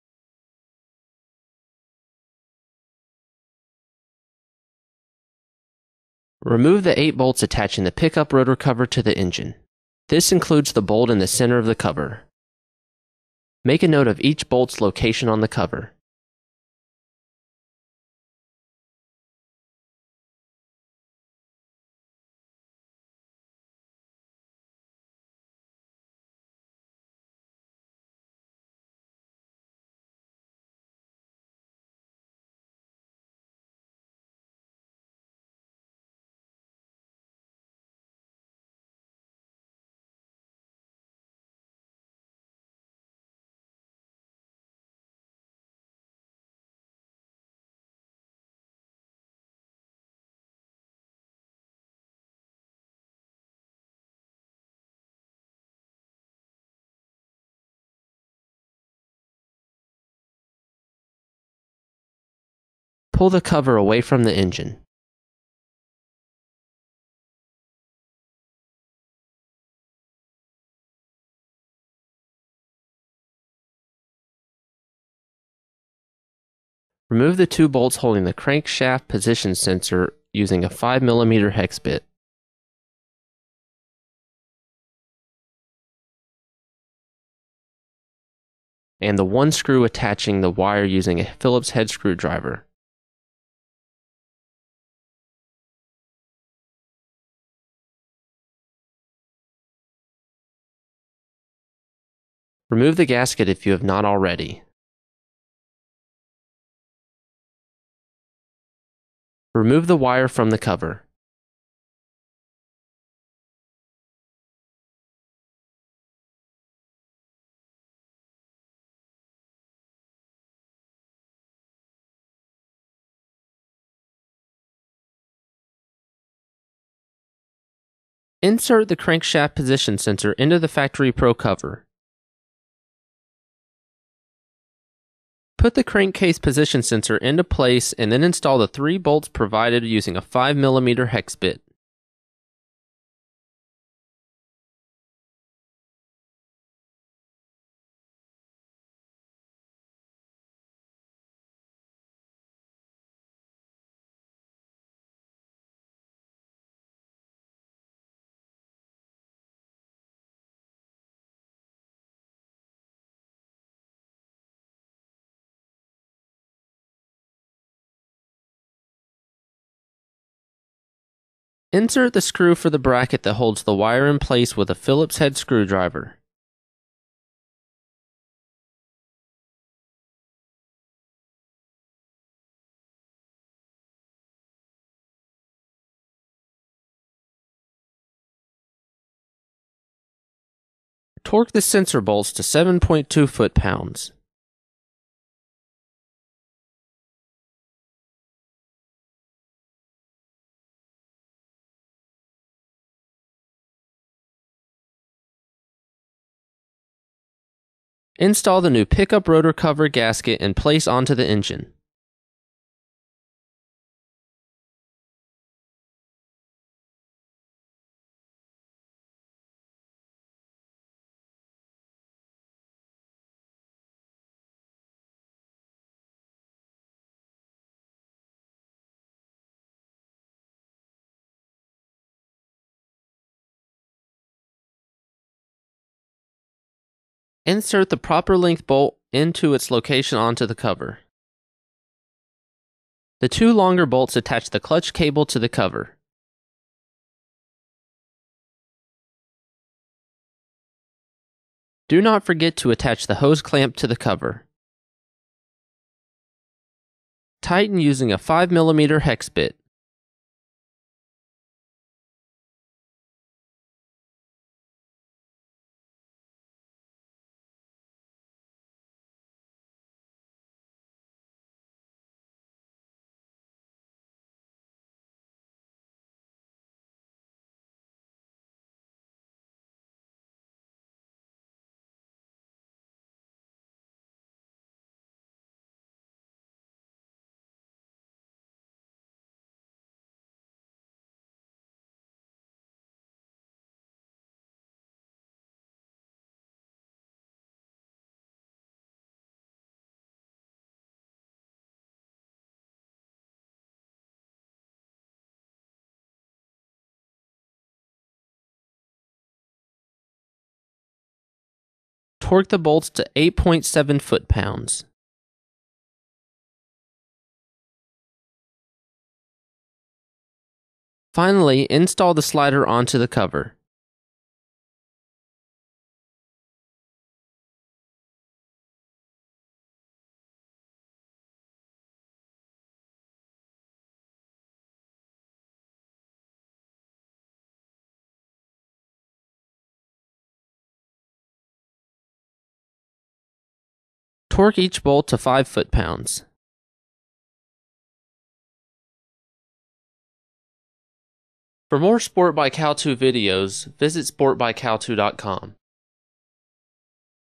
Sportbikehowto.com - How-to Install Pickup Coil Cover R6
How-to install a Facotry Pro pickup coil cover (aka oil pump cover) on a Yamaha R6